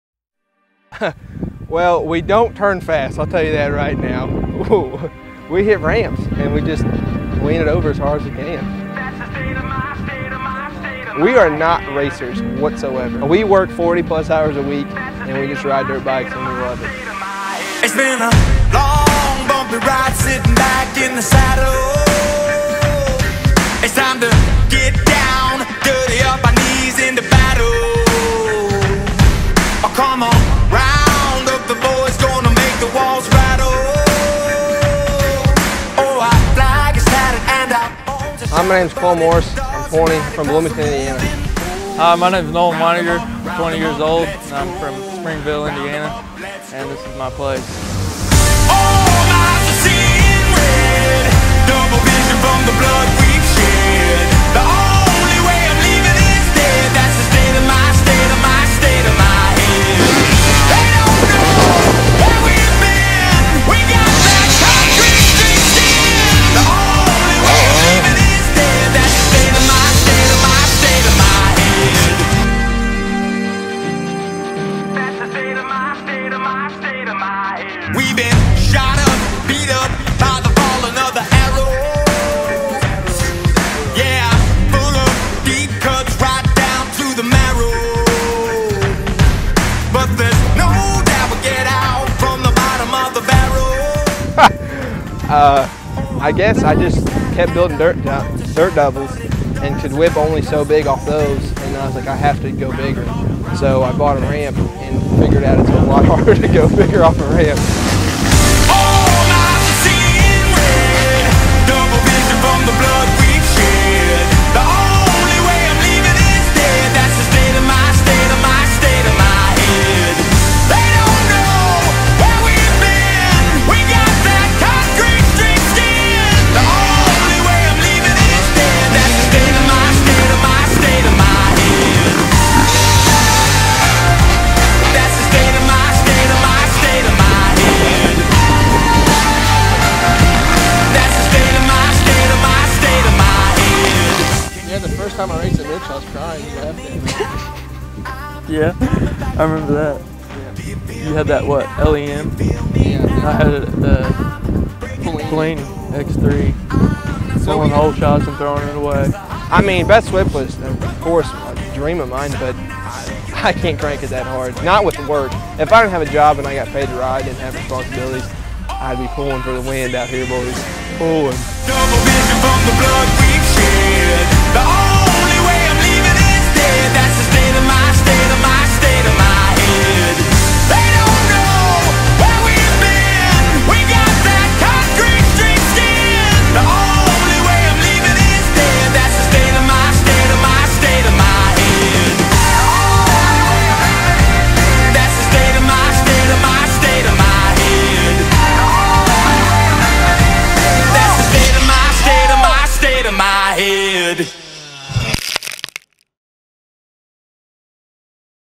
Bloomington Freeride 2015
Local MX riders Cole Morse and Nolan Wininger give us an inside look  at their backyard playground.